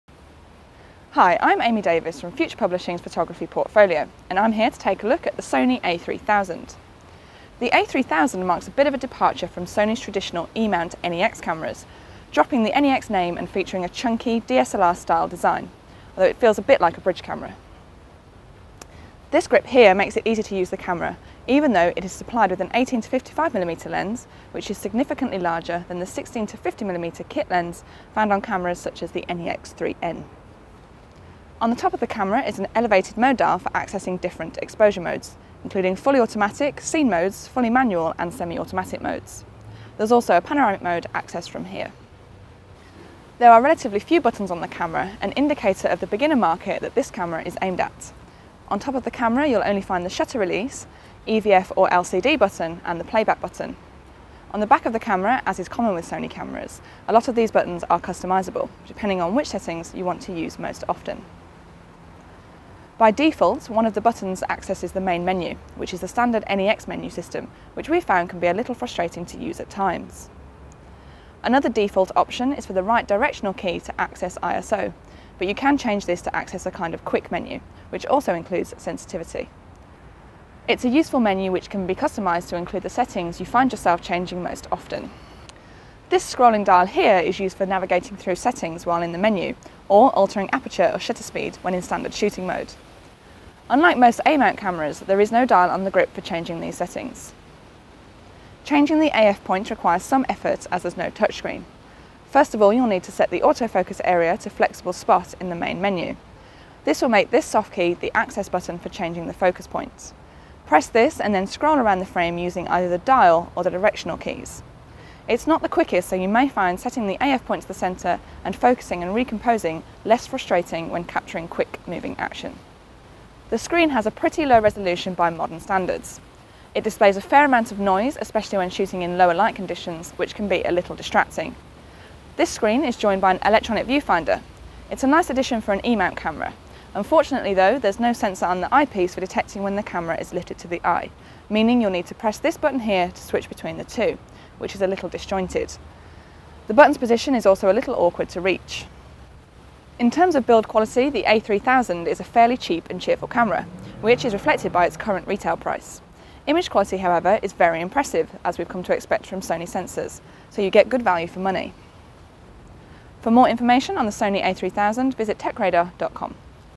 Sony a3000 review
The Sony Alpha a3000 combines the Alpha and NEX systems into a new entry level camera. It's got the styling and size of an Alpha DSLT, but uses the E-mount and mirrorless design of the NEX range.

Here Amy Davies takes a look at what this camera has to offer. For more information and the full review check out